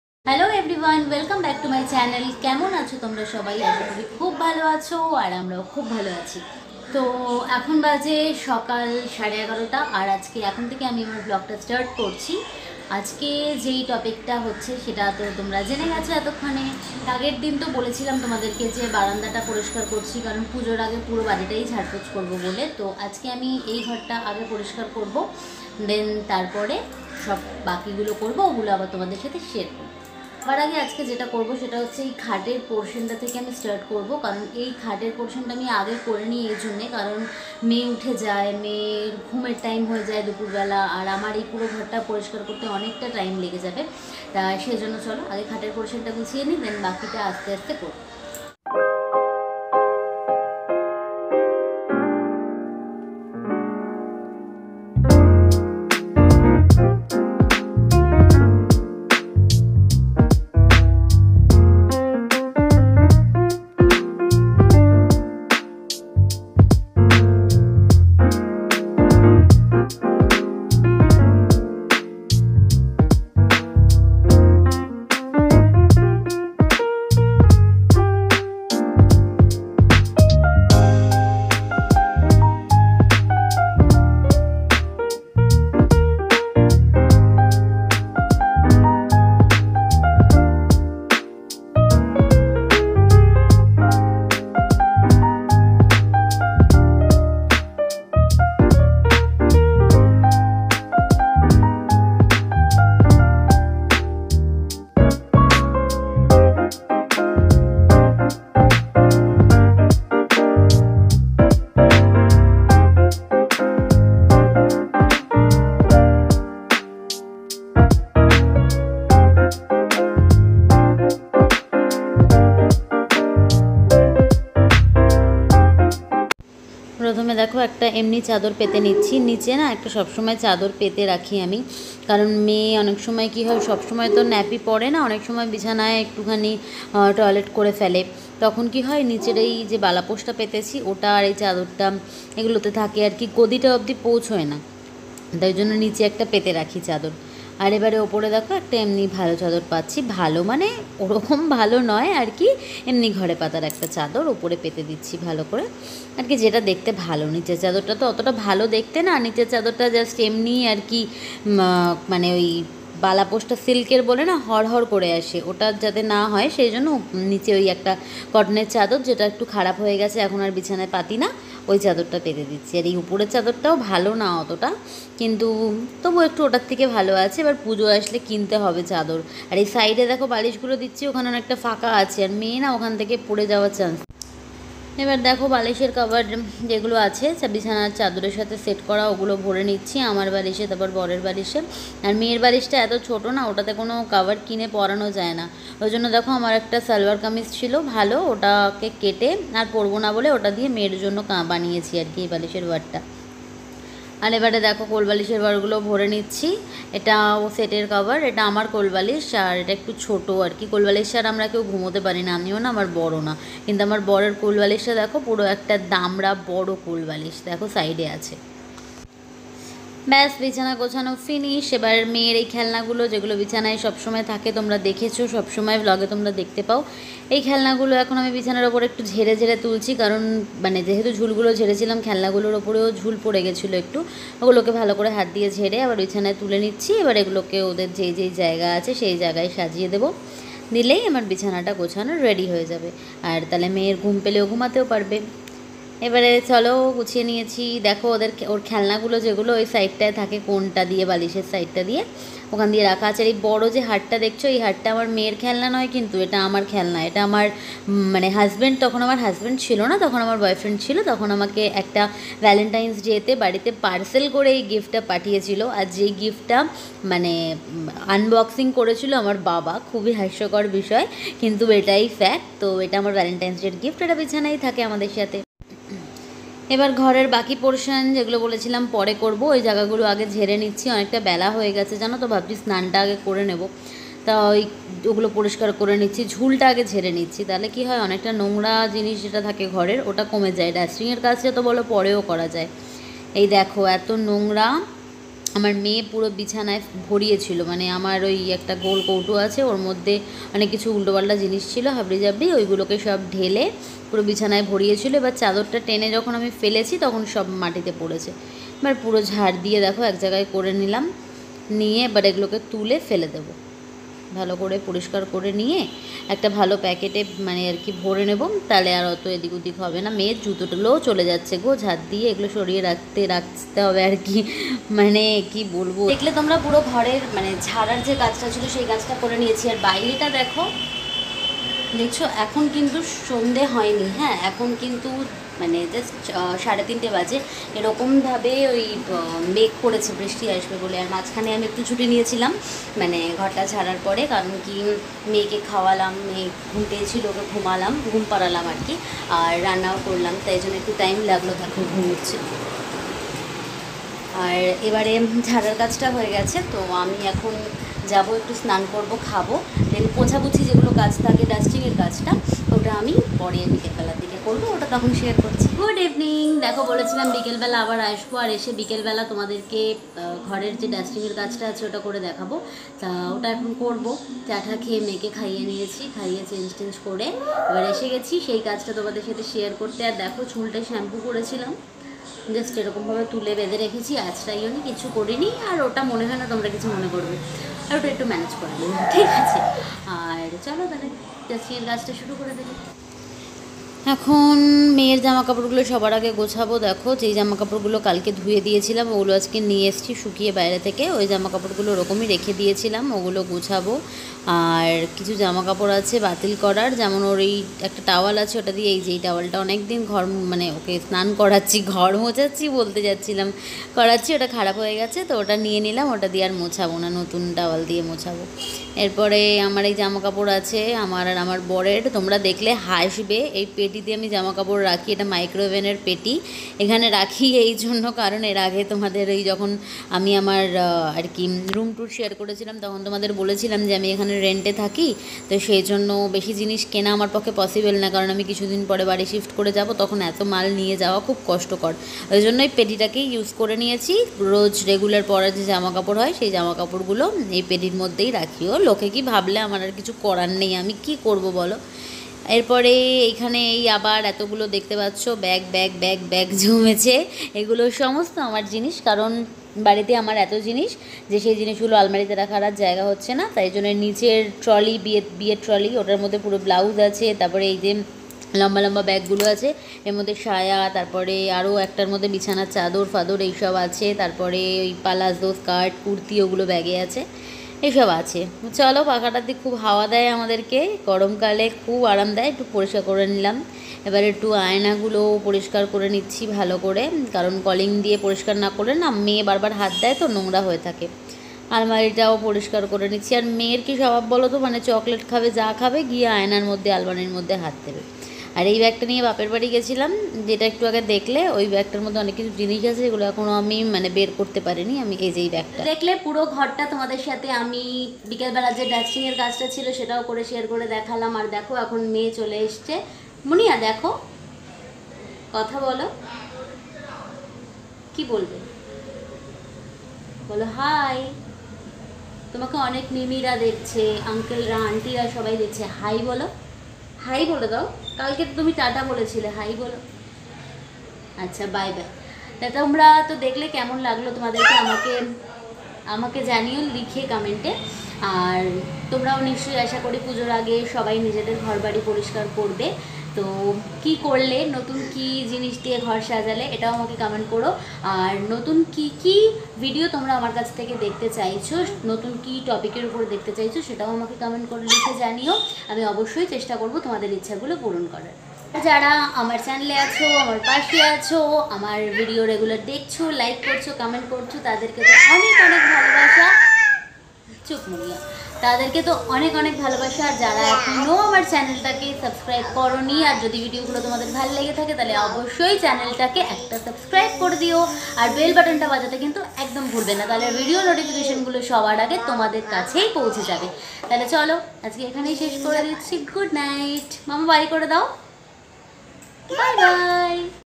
||CLEANING MY ROOM BEFORE DURGA PUJA|| INDIAN VLOGGER SAMARPITA||
this  video is about my room cleaning.